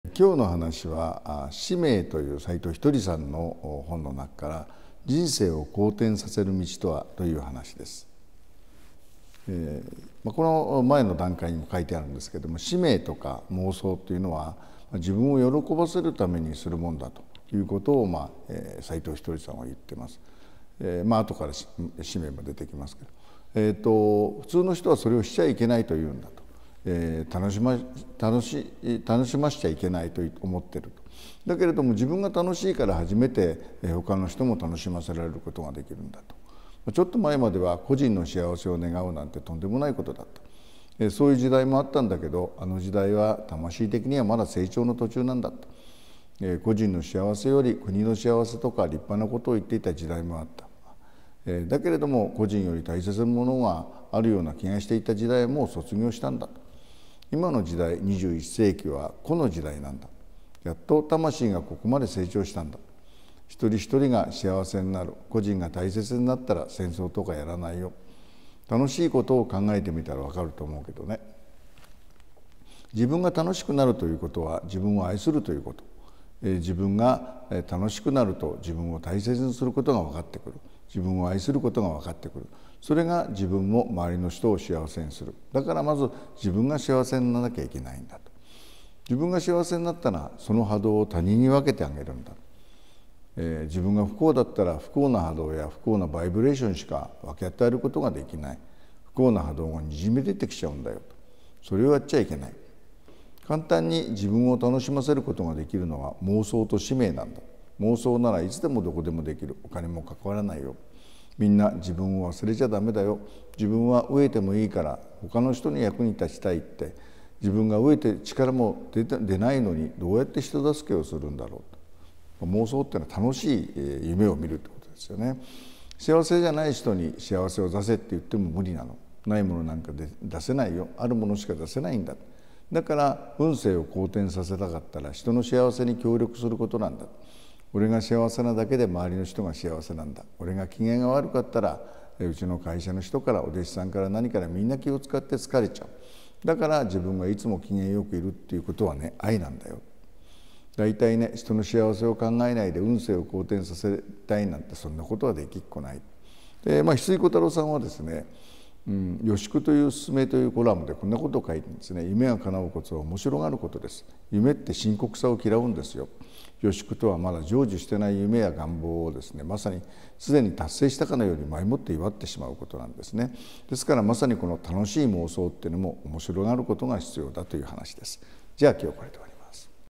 【人生を好転させる道とは】（斎藤一人）
1.「使命」でも「妄想」でも、自分を喜ばせるためにするもんだって思うじゃない？
＊ふつうの人は、それを「いけない」って言うんだよ。
＊自分を「楽しませちゃいけない」と思っているんだよ。
＊だけど、自分が楽しいから、初めて他人も楽しませられるんだよ。

2.ちょっと前までは、個人の幸せを願うなんて、とんでもないことだ、って、そういう時代もあったけど、あの時代は、魂的には、まだ成長の途中なんだよ。
＊個人の幸せより、「国の幸せ」とか、立派なことを言っていた時代もあった。
＊個人より大切なものがあるような気がしていた時代は、もう卒業したんだよ。

3.いまの時代、21世紀は、個の時代なんだよ。
＊やっと魂が、ここまで成長したんだよ。
＊1人ひとりが幸せになる、個人が大切になったら、戦争とかやらないよ。
＊楽しいことを考えてみたらわかると思うけどね。

4.「自分が楽しくなる」ということは、「自分を愛する」ということなの。＊自分が楽しくなると、「自分を大切にする」ことがわかってくる。
＊「自分を愛する」ことがわかってくる。
＊それが、自分も、周りの人も幸せにするんだよ。だから、まず自分が幸せになるんだよ。
5.自分が幸せになったら、その波動を他人に分け与えるの。
＊自分が不幸だったら、不幸な波動や、不幸なバイブレーションしか、分け与えることができない。
＊不幸な波動が、にじみ出ちゃうの。
＊それをやっちゃいけないんだよ。

6.簡単に、自分を楽しませることができるのは「妄想」と「使命」なんだよ。
＊「妄想」なら、いつでもどこでもできる。お金もかからないんだよ。
7.みんな、自分を忘れちゃダメだよ。＊「自分は飢えてもいいから、他人の役に立ちたい」って。
＊自分が飢えて力も出ないのに、どうやって人助けするんだろう。

8.幸せじゃない人に、「幸せを出せ」って、無理なの。
＊ないものなんか、出せないよ。あるものしか出せないの。
9.だから、運勢を好転させたかったらね、人の幸せに協力することなの。
＊オレが幸せなだけで、周りの人が幸せなんだ。
＊オレが機嫌が悪かったらね、ウチの会社の人からお弟子さんから何から、みんな、気を遣って疲れちゃうの。
＊だから、自分がいつも機嫌良くいる、っていうことはね、「愛」なの。

10.だいたいね、人の幸せを考えないでね、「運勢だけ好転させたい」って、そんなことはできっこないんだよ。＊いま、自分が何も変わらないで、行動を変えないで、「結果だけ変えたい」っていうのは、無茶なんだよ。
＊まず、変えることは、楽しい妄想で自分を幸せにすること。
＊その次に、「自分の使命」に燃えて他人の幸せに協力すること。
＊これが、神が望む「人生を好転させる道」なんだよ。

◆（ひすいこたろう）
＊「予祝（よしゅく）のススメ」というコラムで、こう書いている。＊『夢が叶うコツは面白がることです。夢って「深刻さ」を嫌うんです。』
＊予祝とは、まだ成就していない夢や願望を、すでに達成したかのように、前もって祝ってしまうこと。

◆まさに、楽しい妄想も、面白がることが必要。＊だから、深刻さを嫌う。
＊深刻ではなく、面白がることが、人生を好転させる。